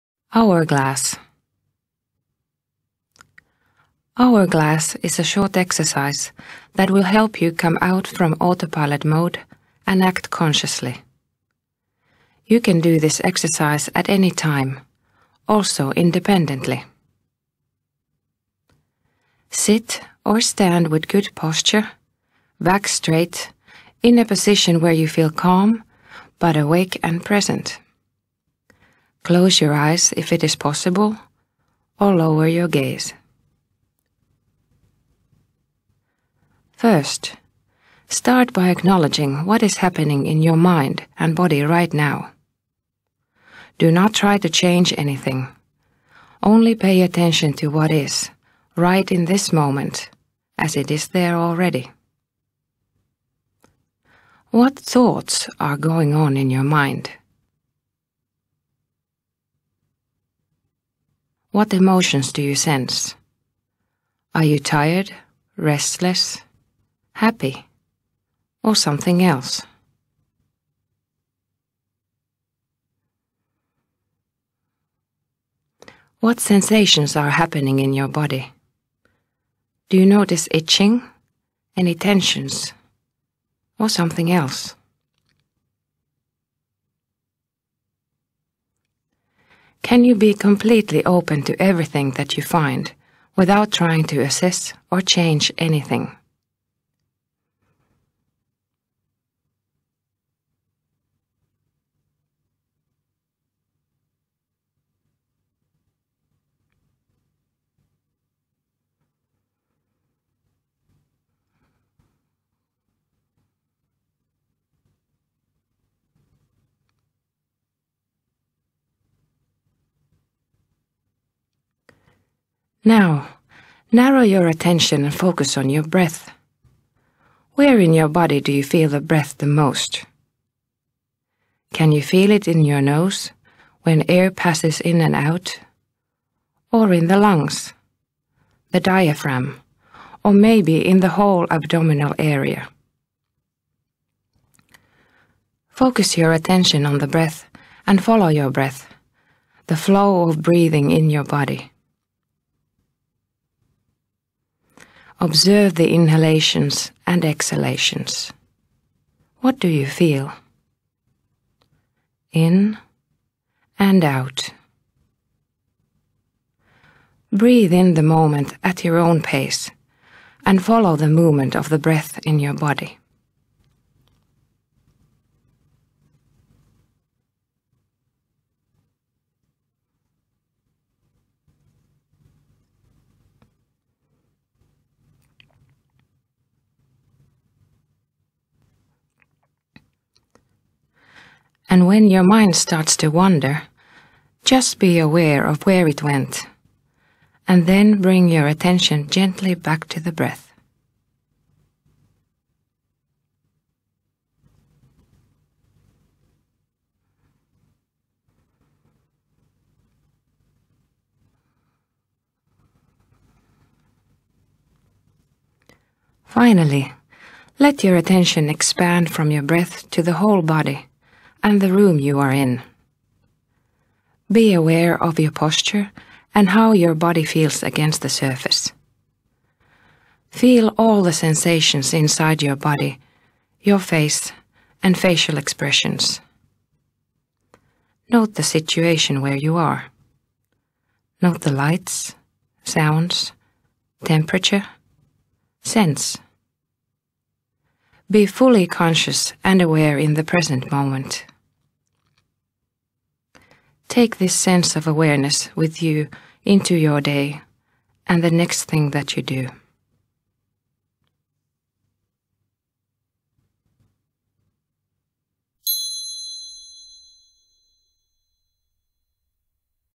Mindfulness meditation 'Hourglass'
Want to try out some mindfulness exercises for yourself? Just put the headphones on (if you wish!) and start the video. And then – breathe.